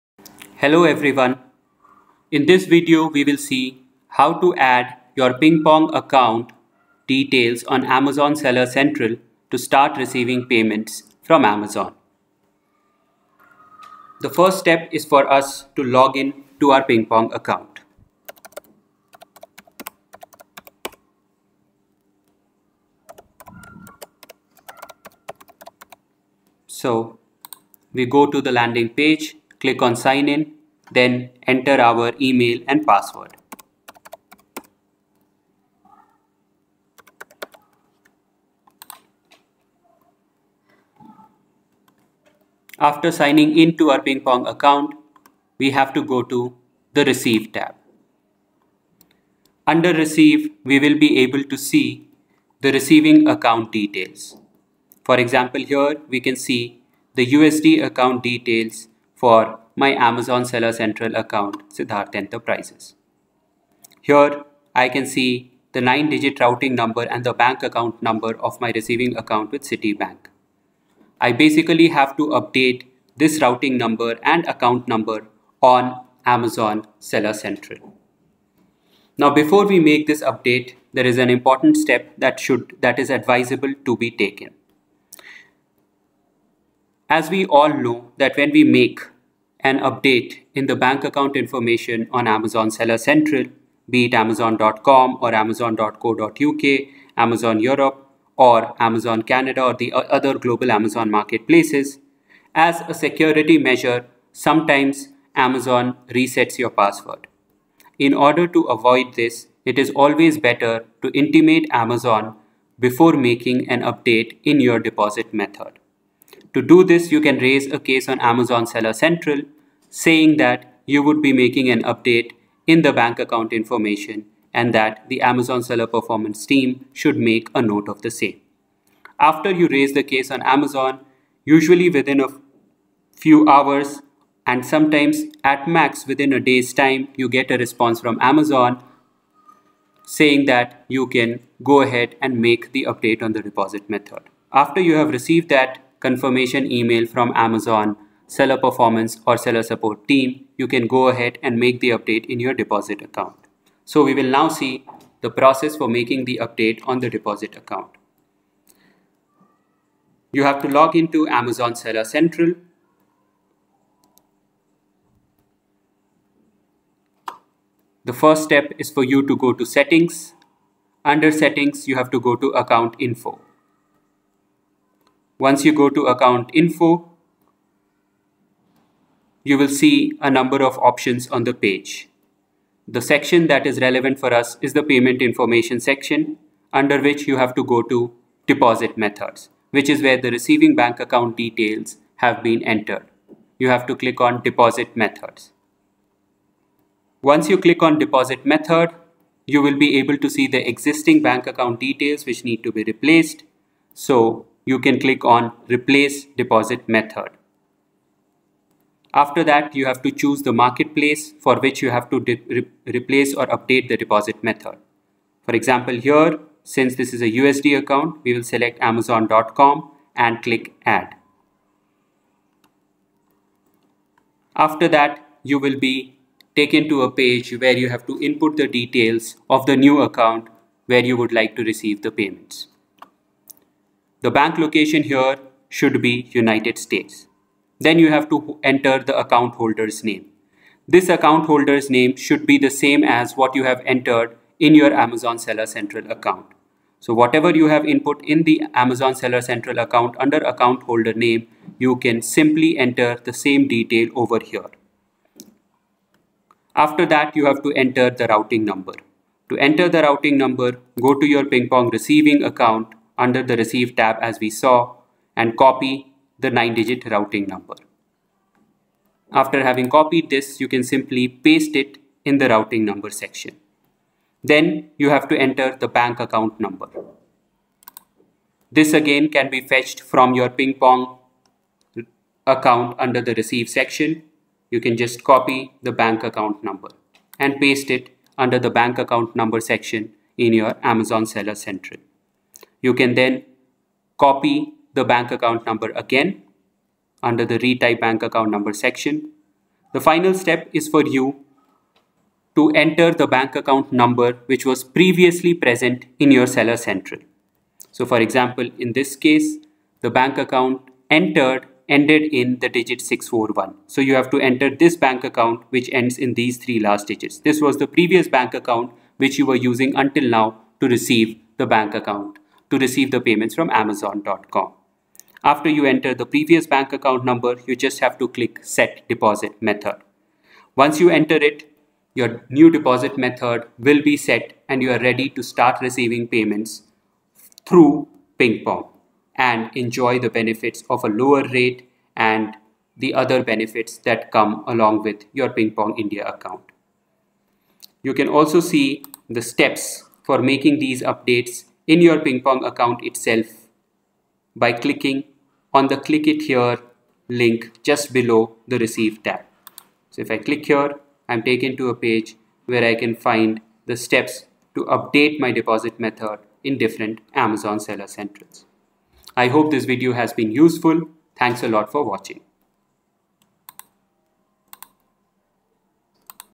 How to add PingPong Account on Amazon seller account
Add your PingPong Account detail to Amazon seller account to receive payments from Amazon marketplace.